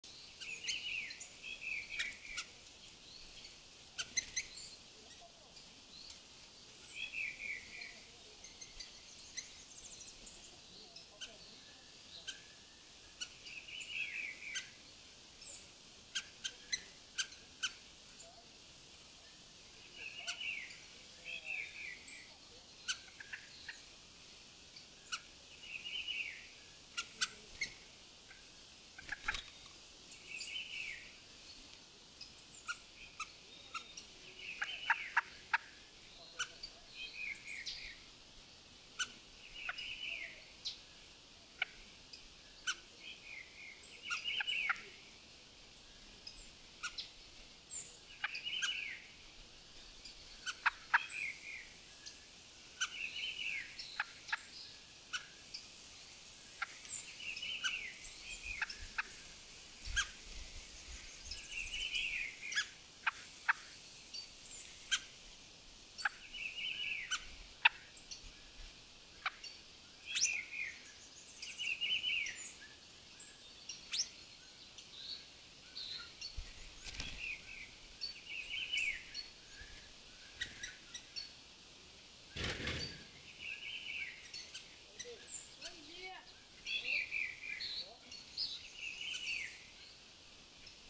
Sounds of nature, image of a brazilian "Sabiá Laranjeira" bird
PUT YOUR VOLUME LOUDER!

WEAR HEADPHONES!!!

One minute of sounds of nature with the image of a Brazilian Sabiá Laranjeira eating from my bird feeder.